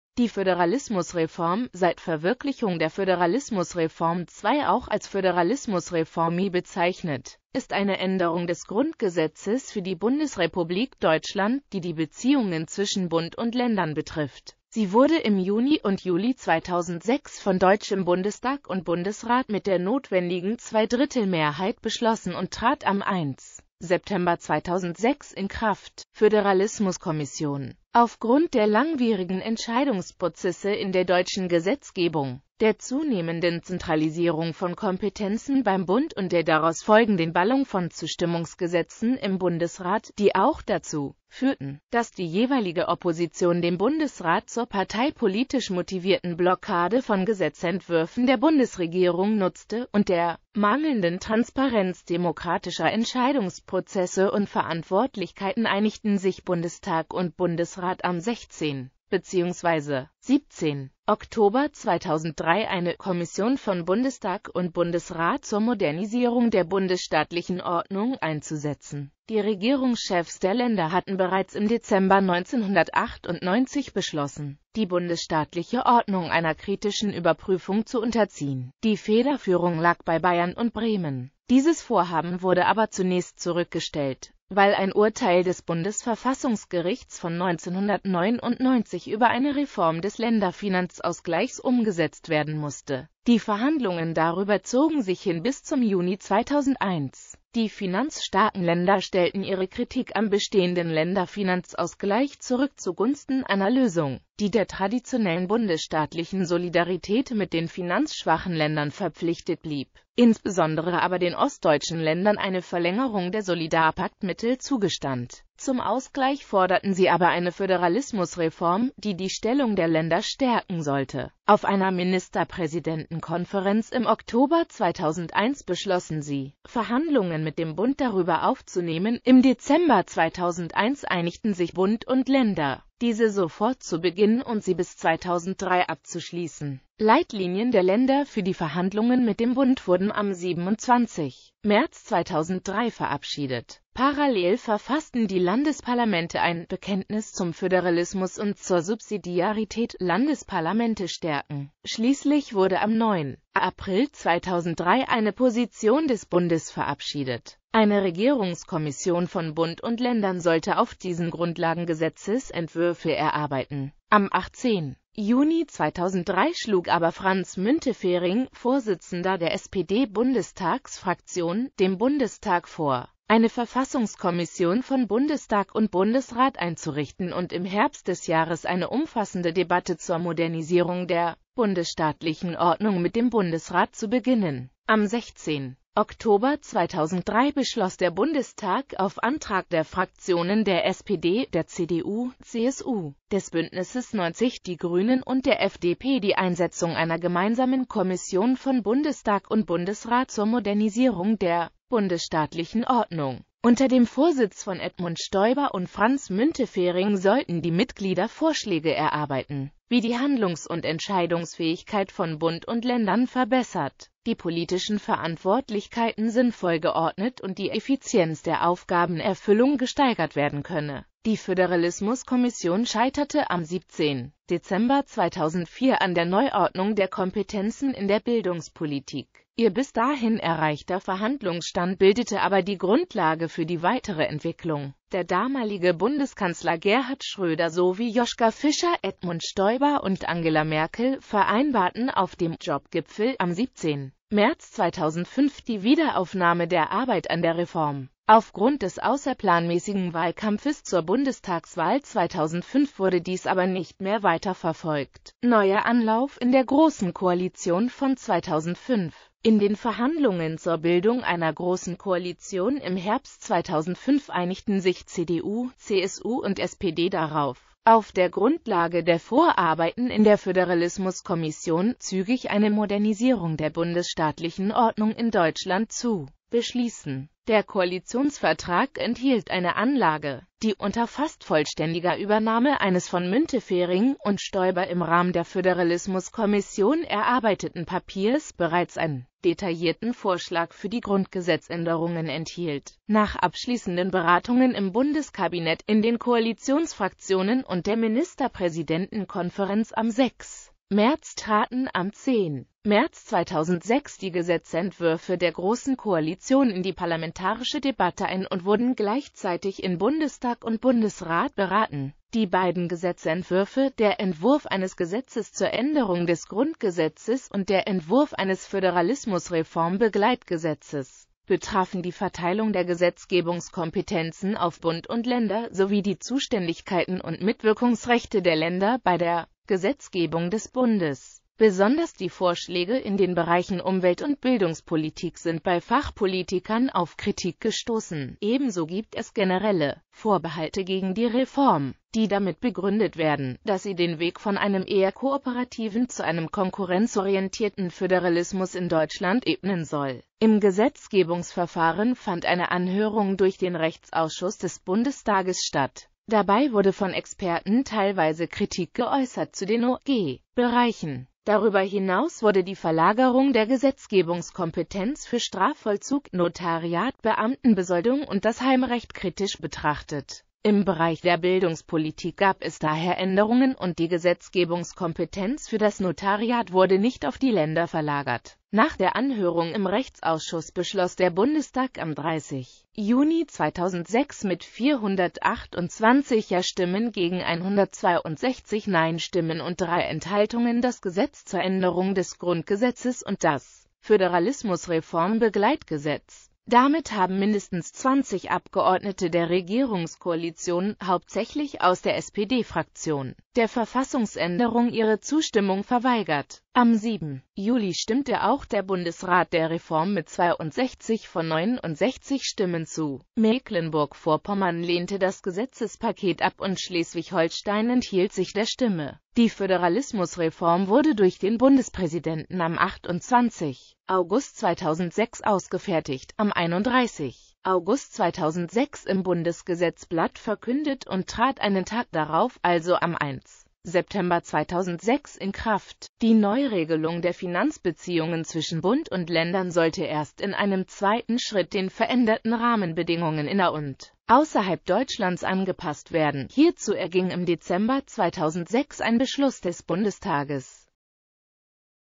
Föderalismusreform
Vielen Dank für Ihre Unterstützung:

Die Föderalismusreform, seit Verwirklichung der Föderalismusreform II auch als Föderalismusreform I bezeichnet, ist eine Änderung des Grundgesetzes für die Bundesrepublik Deutschland, die die Beziehungen zwischen Bund und Ländern betrifft.Sie wurde im Juni und Juli 2006 von Deutschem Bundestag und Bundesrat mit der notwendigen Zweidrittelmehrheit beschlossen und trat am 1.September 2006 in Kraft.